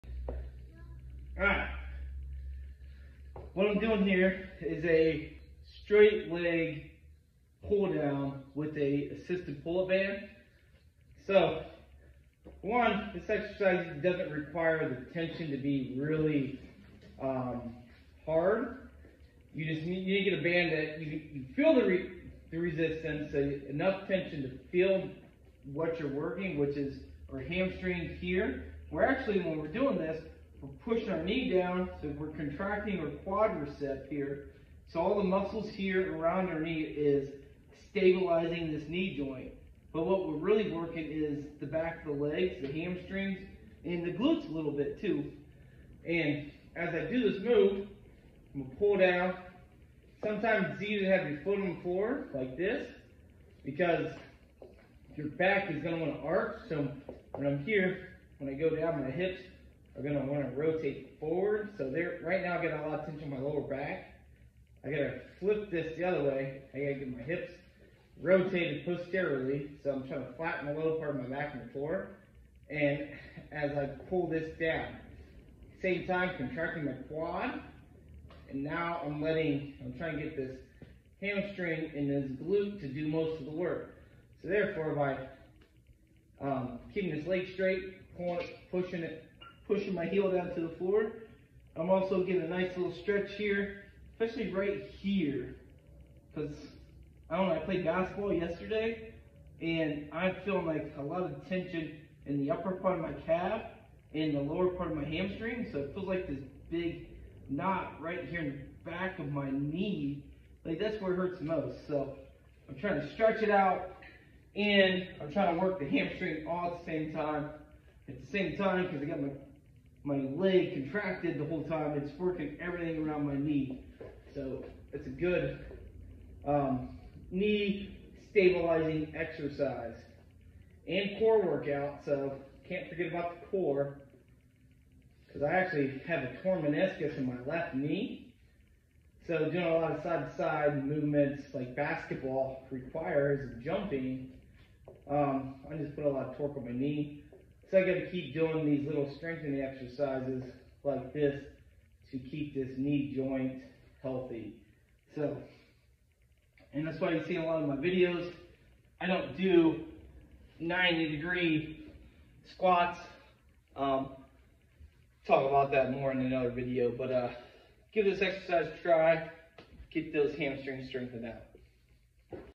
Knee strengthening exercise
Straight leg pull downs with a assisted pull up band.
I’m doing this exercise to strengthen the muscles around my knee. Yes, the prime mover is the hamstring but I am contracting my entire quadriceps and hamstring throughout the entire movement.
I actually have a torn menicus that has been torn for about 10 years now so I have to make sure the mud around my knees are strong.
Give this exercise a try and tell me what you think.